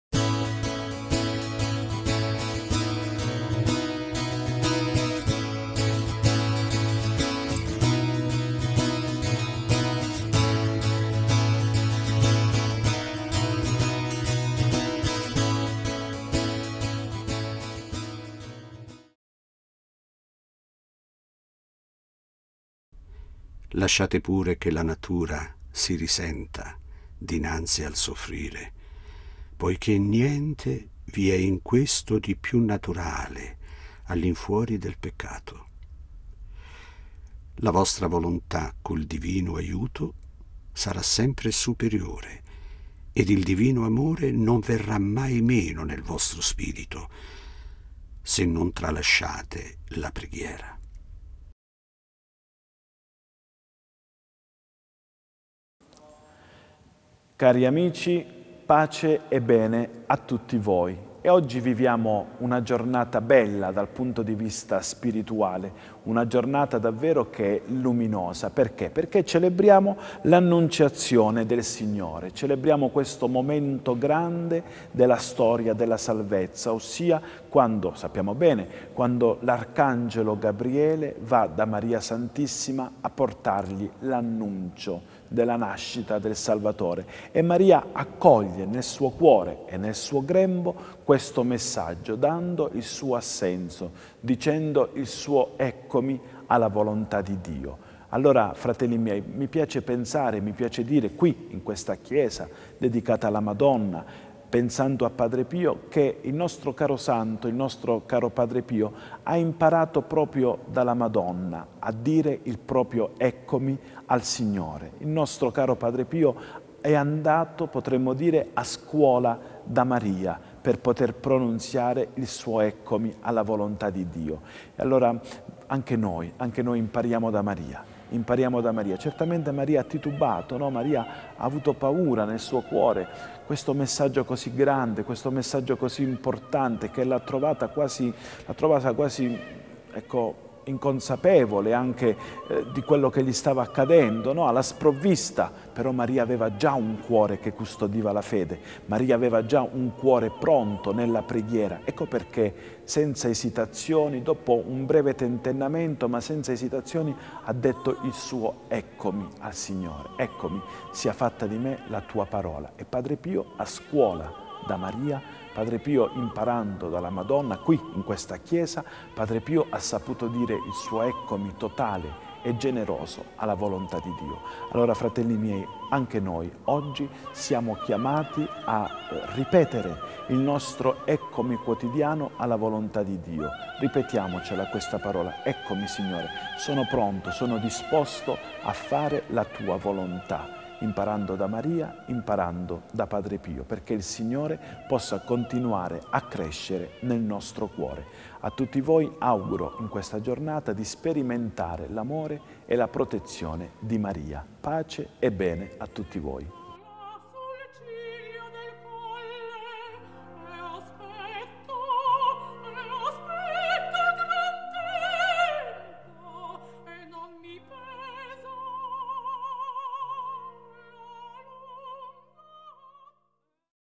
Buona Giornata con Padre Pio 25 Marzo 2020
La Frase di Padre Pio:
"Lasciate pure che la natura si risenta dinanzi al soffrire, poiché niente vi è in questo di piú naturale all’infuori del peccato; la vostra volontà, col divino aiuto, sarà sempre  superiore  ed il divino amore non verrà  mai  meno nel vostro spirito, se non tralasciate la preghiera"